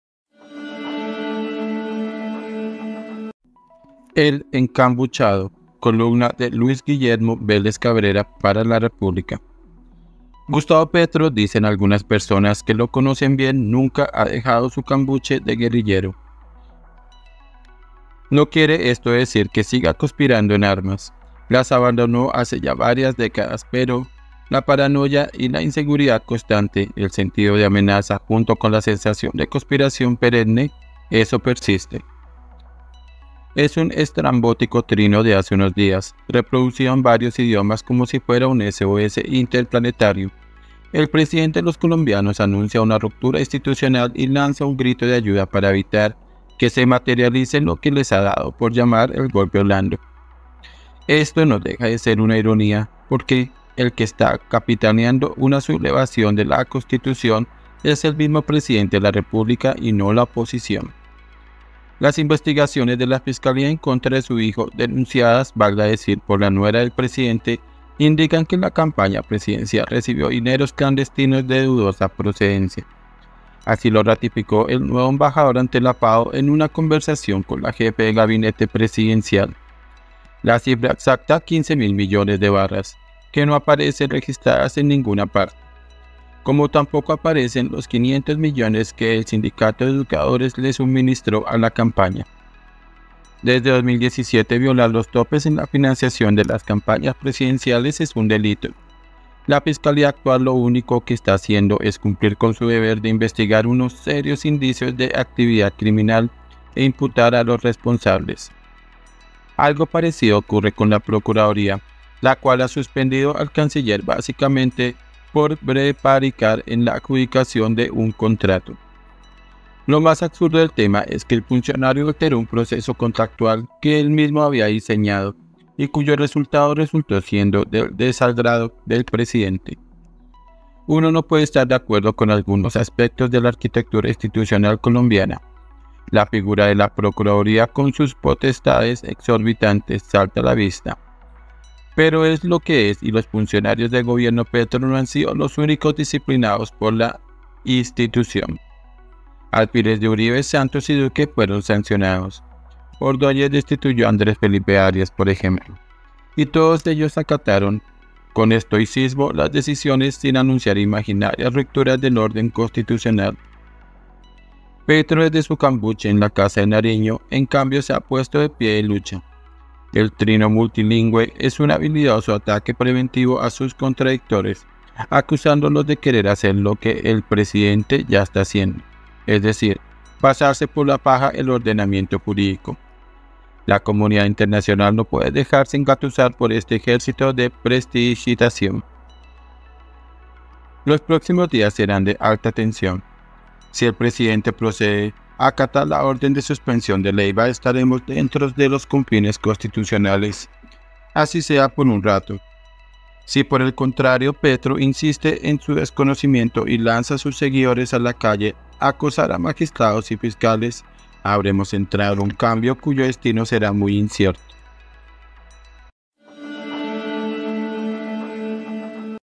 EL ENCAMBUCHADO 🟡 Luis Guillermo Vélez Cabrera
Gustavo Petro, dicen algunas personas que lo conocen bien, nunca ha dejado su cambuche de guerrillero. No quiere esto decir que siga conspirando en armas. Las abandonó hace ya varias décadas, pero la paranoia y la inseguridad constante, el sentido de amenaza, junto con la sensación de conspiración perenne, eso persiste.
columna LUIS GUILLERMO VÉLEZ CABRERA para LA REPÚBLICA